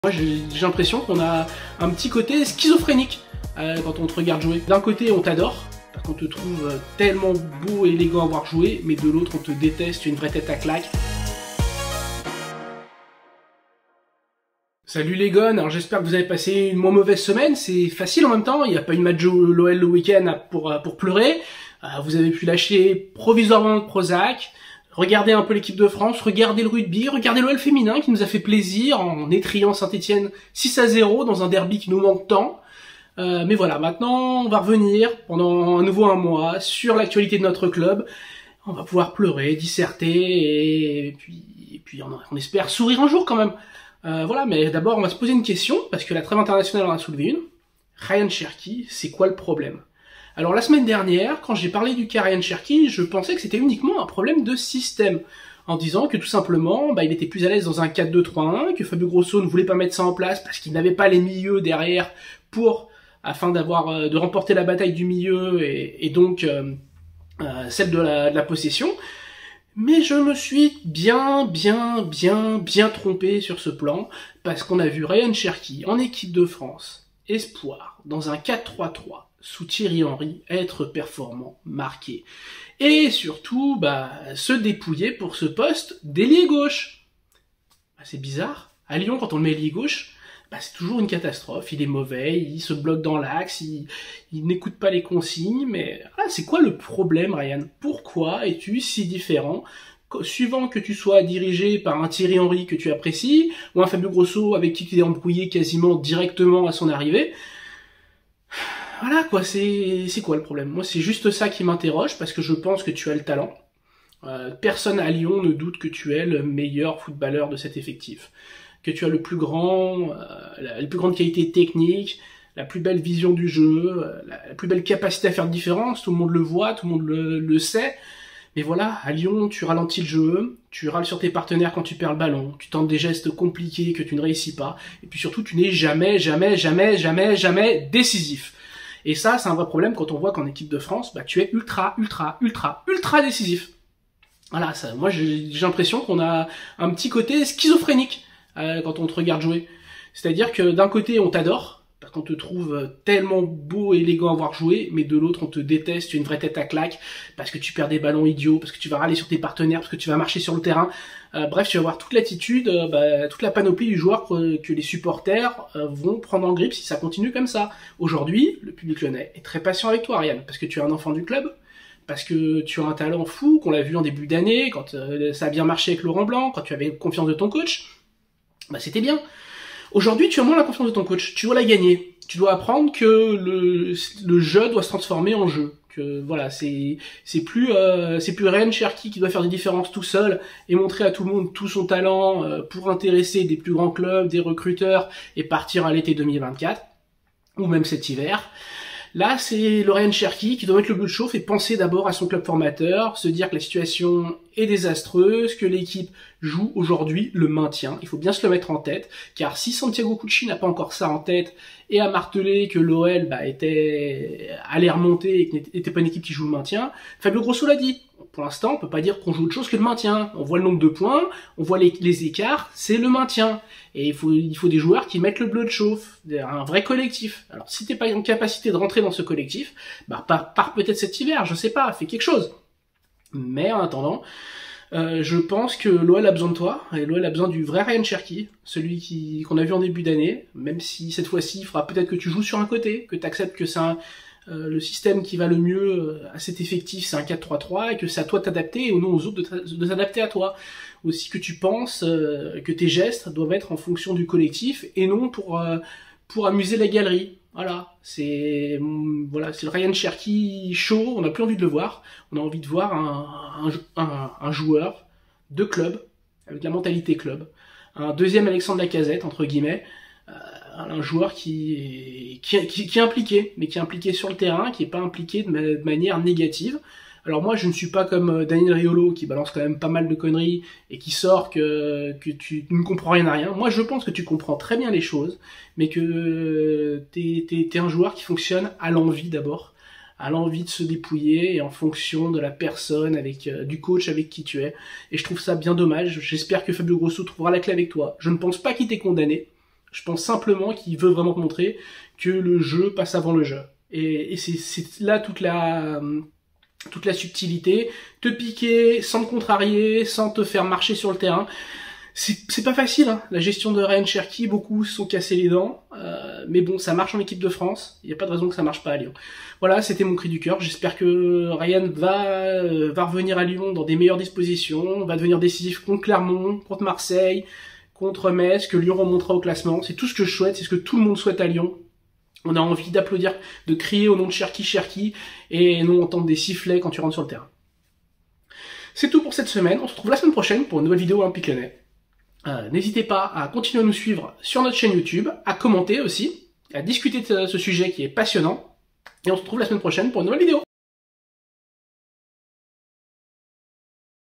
OL : Rayan Cherki, c'est quoi le problème ?
Et si il y avait deux Rayan Cherki ? Un, étincelant avec les Espoirs et un autre, beaucoup moins bon sous le maillot lyonnais...